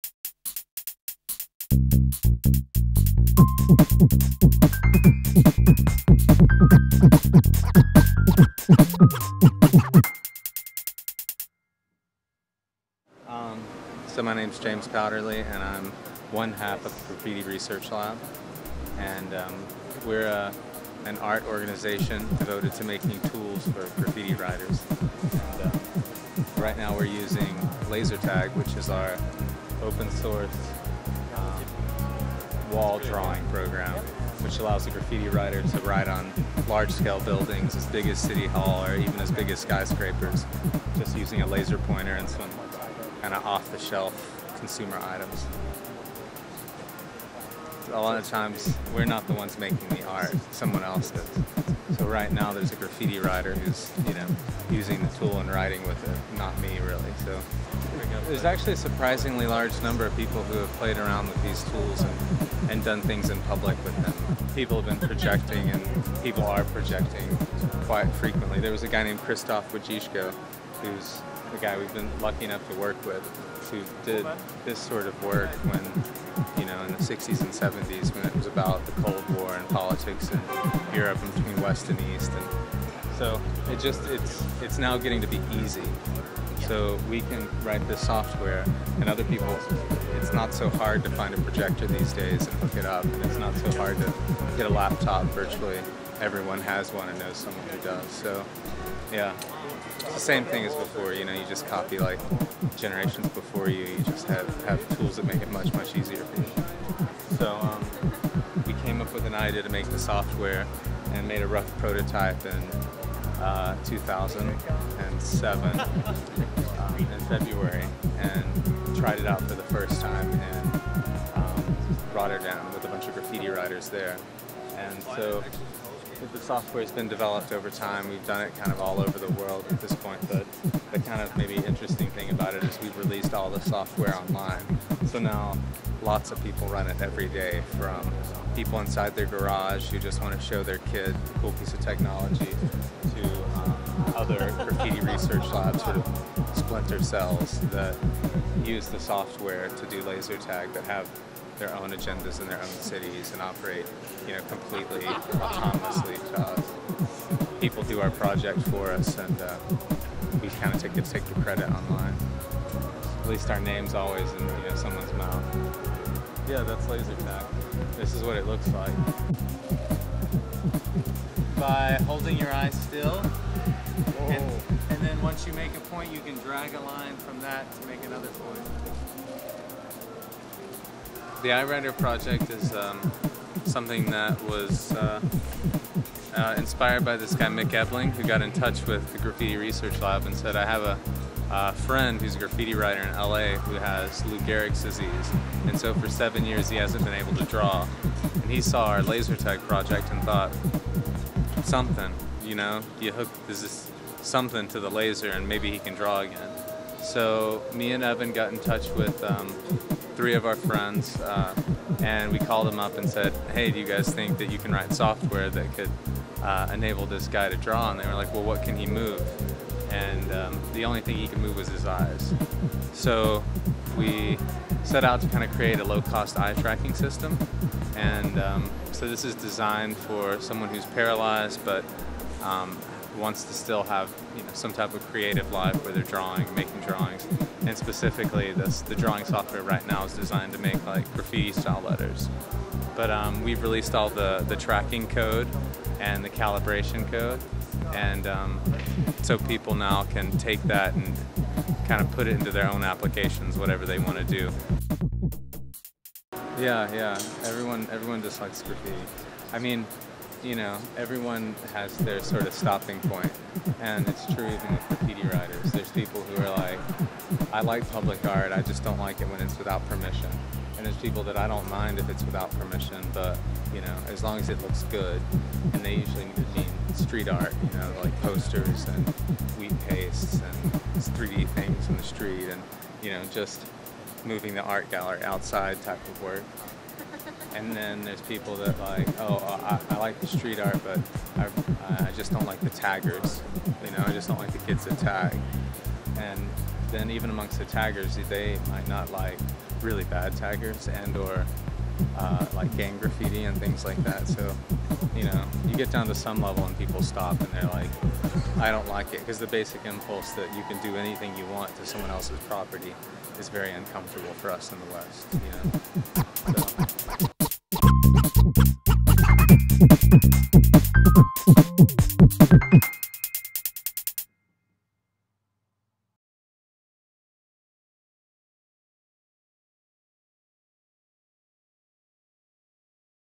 Veracity: Graffiti Research Lab
James Powderly talks about the projects "Laser Tag" & "Eye Writer" during CAFKA.09: Veracity.